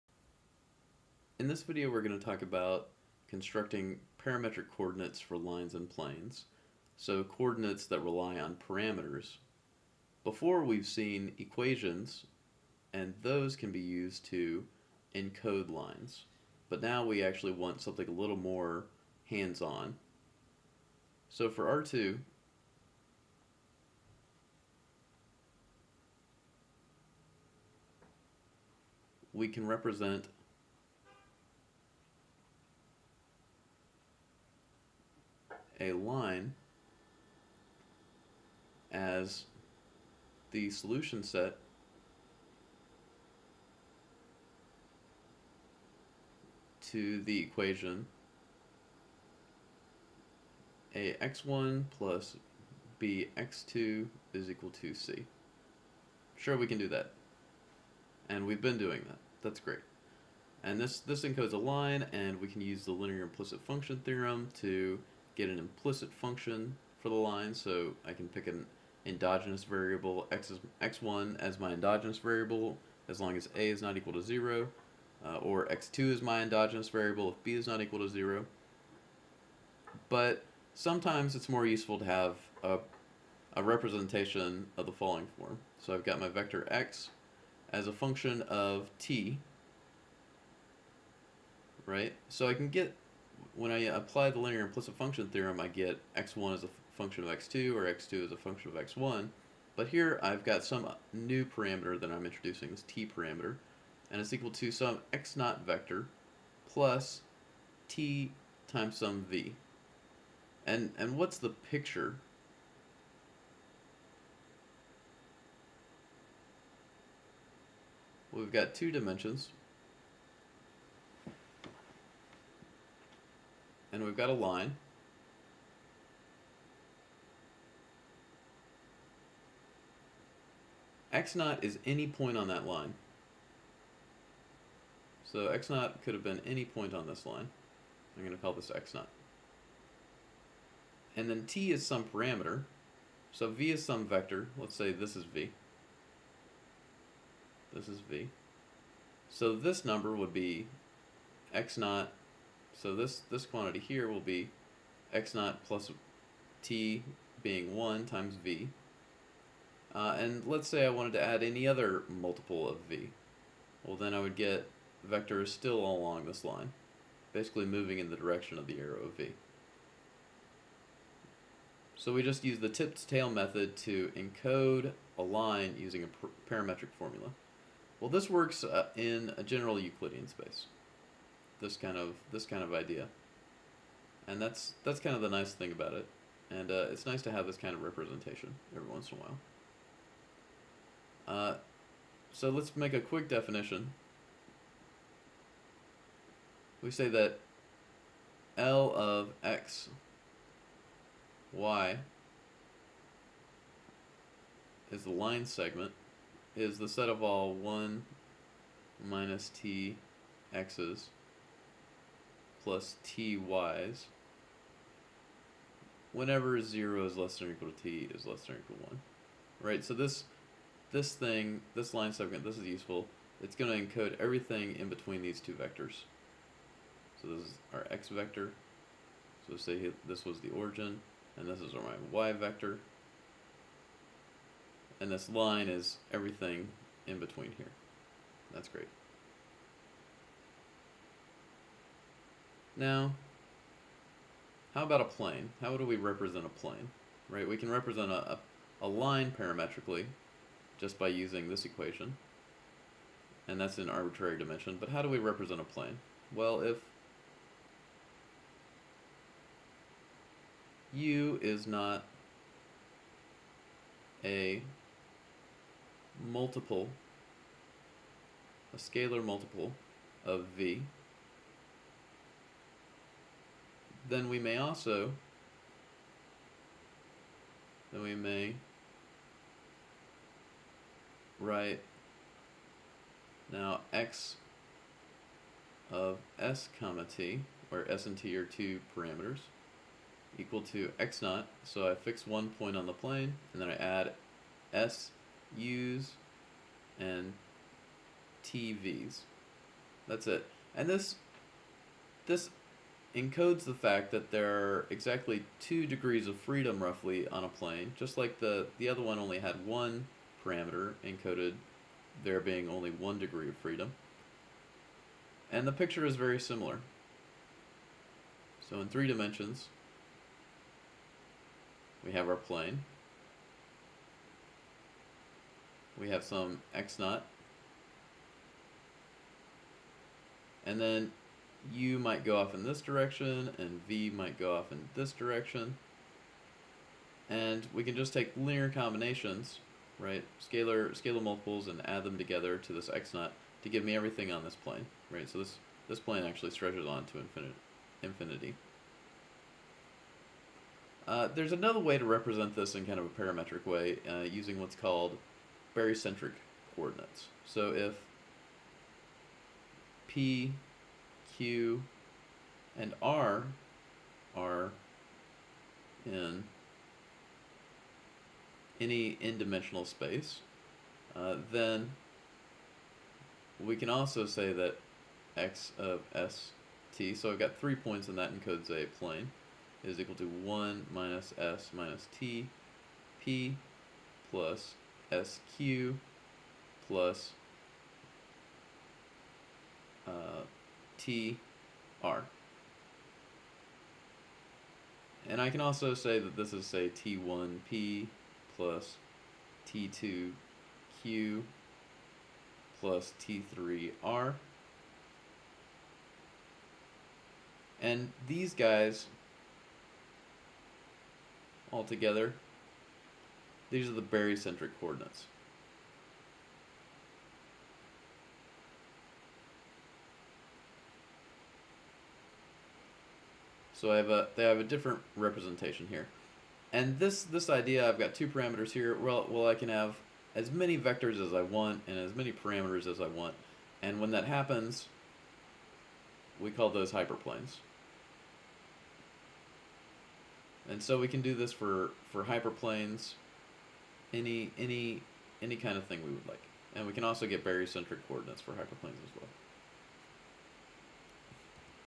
Euclidean Spaces Lecture 2 Part 3: Parametric Equations
We show how lines, planes, and hyperplanes can be defined in terms of parameters.